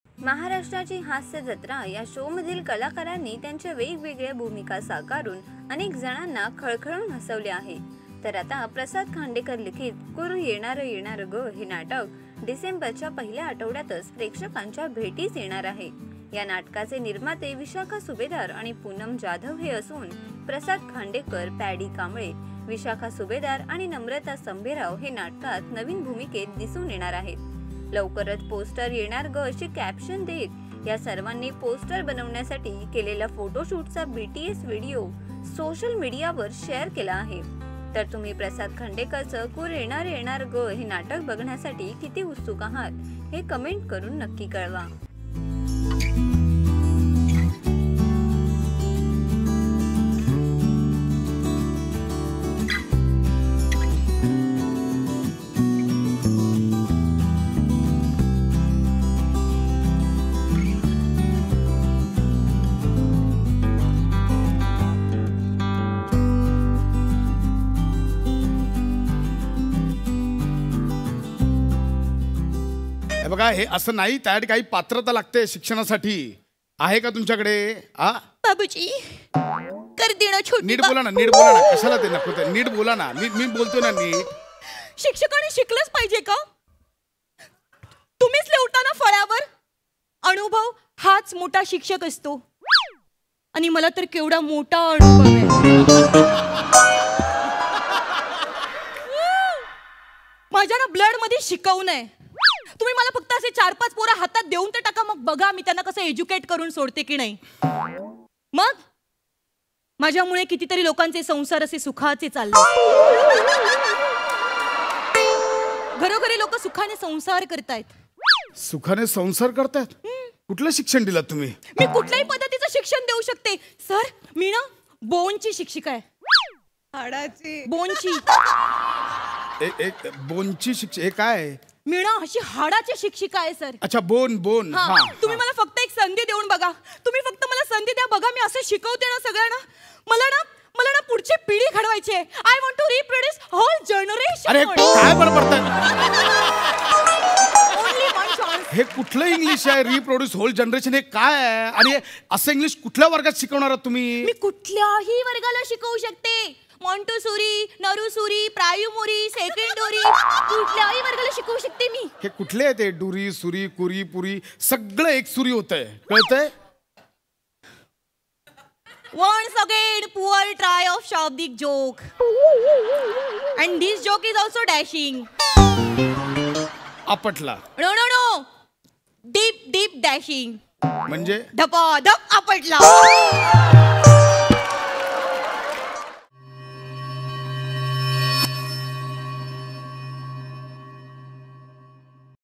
प्रसाद खांडेकर झाला दिग्दर्शक | Prasad Khandekar | Maharashtrachi Hasyajatra | itsmajja
अभिनेता प्रसाद खांडेकरचं कुर्रररर नाटक लवकरच प्रेक्षकांच्या भेटीस येणार